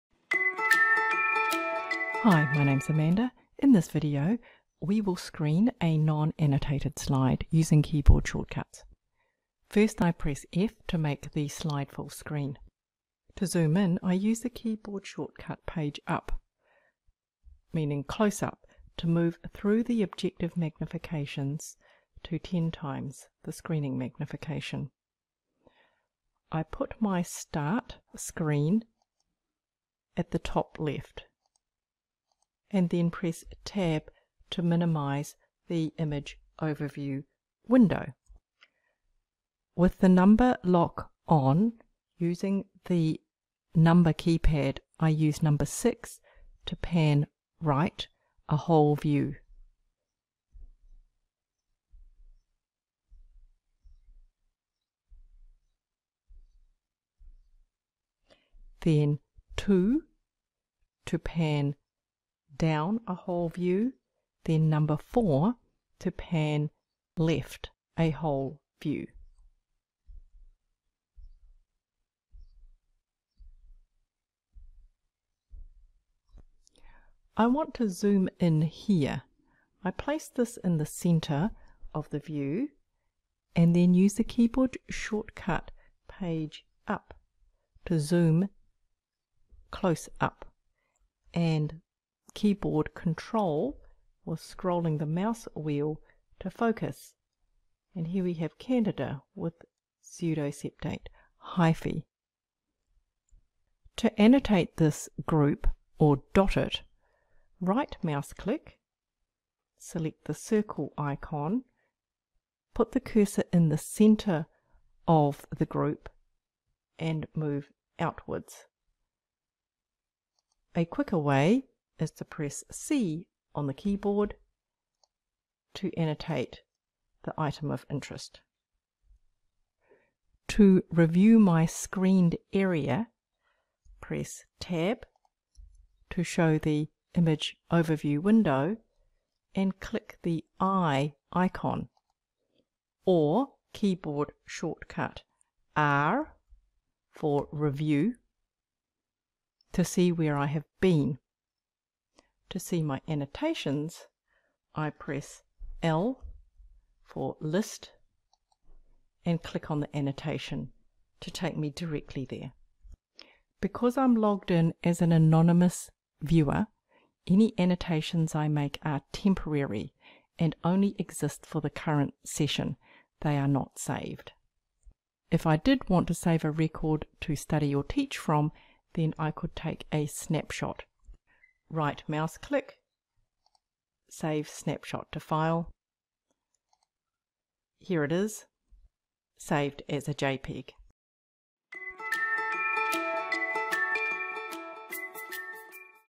4 screening non annotated slide
Table of contents, click on the blue time stamp to jump directly to content
0:09 Full screen
0:12 Zoom
0:31 Minimise image overview window
0:40 Pan a whole screen right
0:58 Pan a whole screen down
1:04 Pan a whole screen left
1:16 Zoom
1:30 Focus
1:43 Annotate
2:10 Review screened area
2:31 List annotations
2:54 Snapshot